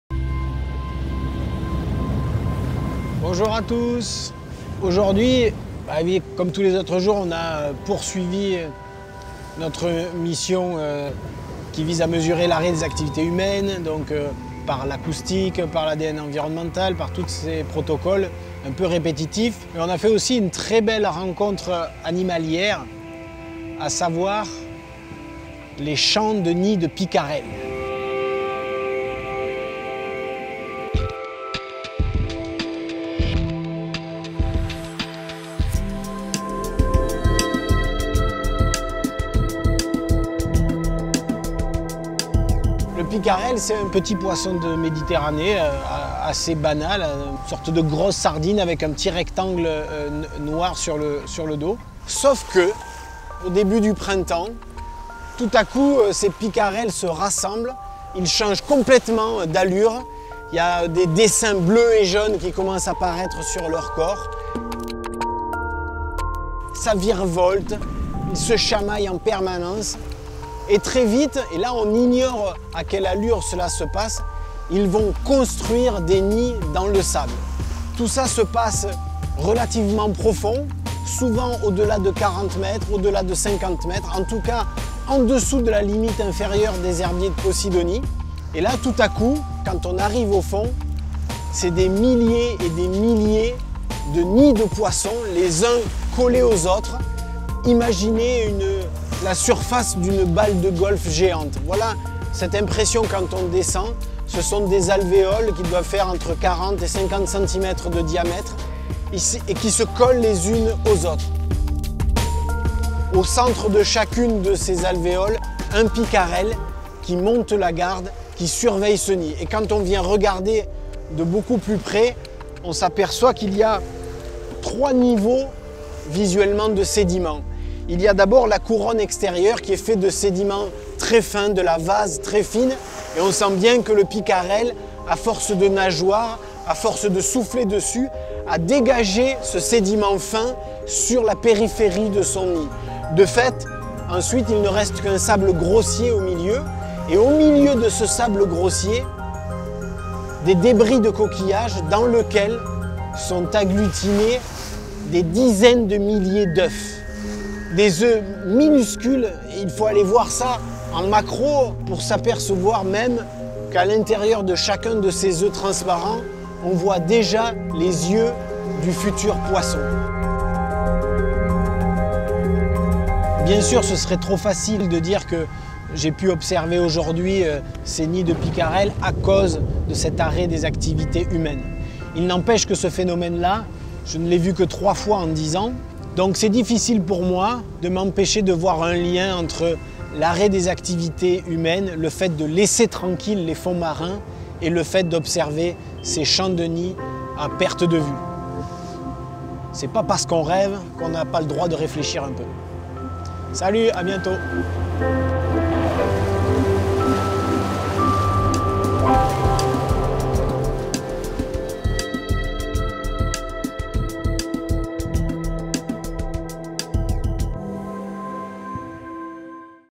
Lockdown & Biodiversity Expedition - EP2
Spring 2020 provided exceptional conditions for studying marine biodiversity due to the lockdown resulting from the COVID-19 pandemic. The halt brought to tourist, sporting and fishing activities resulted in human presence on the coastline being reduced to minimum levels. Founding partner of the Gombessa expeditions, Blancpain is supporting a new mission by underwater biologist and photographer Laurent Ballesta. This project, conducted in the Mediterranean, aims to study the impact on marine vertebrate and invertebrate populations of the cessation of human activities due to the COVID-19 pandemic. Financed by part of the proceeds from the sale of BOC III limited edition watches, it follows on from the Gombessa V expedition that took place in July 2019 off the French Mediterranean coast.